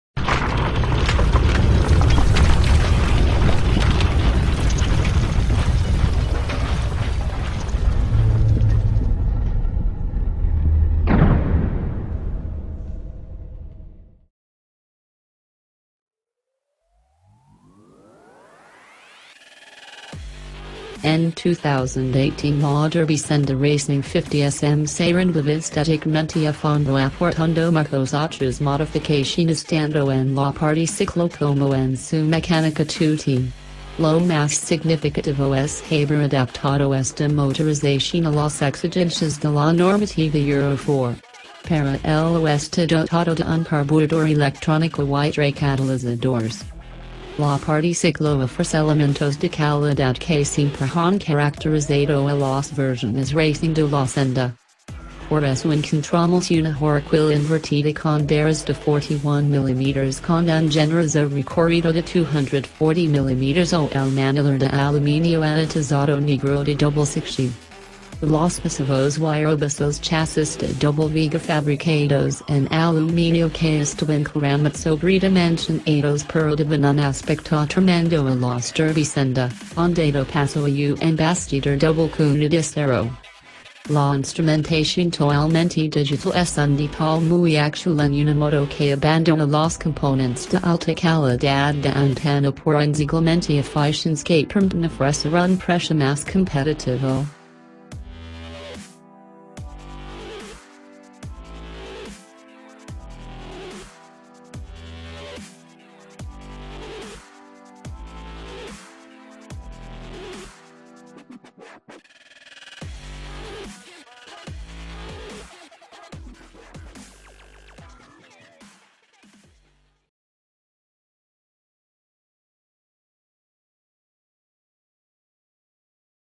Otto Bike l 2018 Senda Racing 50 SM - 2017 EICMA Milan
Otto Bike l 2018 Senda Racing 50 SM - 2017 EICMA Milan-En 2018 la Derbi Senda Racing 50 SM se renueva estéticamente a fondo aportando muchos otras modificaciones tanto en la parte ciclo como en su mecánica 2T. Lo más significativo es haber adaptado esta motorización a las exigencias de la normativa Euro4. Para ello está dotado de un carburador electrónico y tres catalizadores.

██████ GET DAILY UPDATES! NOW! ██████